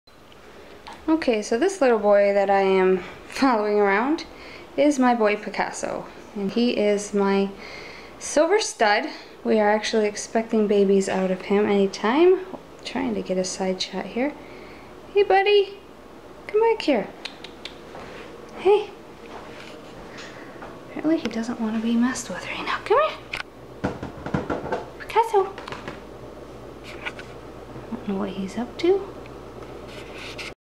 Silver Bengal Stud
Meet Auroralights Picasso, a beautiful work of art and my new stud.